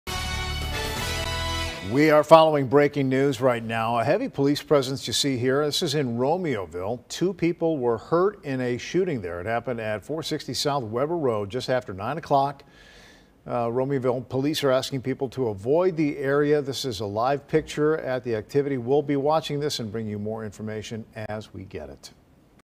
2 people shot at Scene75 Entertainment Center in Romeoville
Two people were shot Friday night at an entertainment center in Romeoville, according to police.